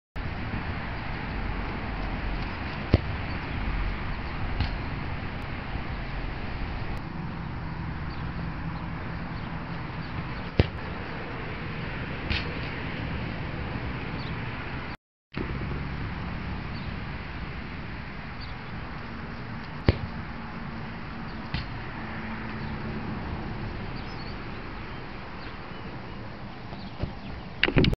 What the...?? 3 post shots in a row
some good 30 yrd shots hit the post.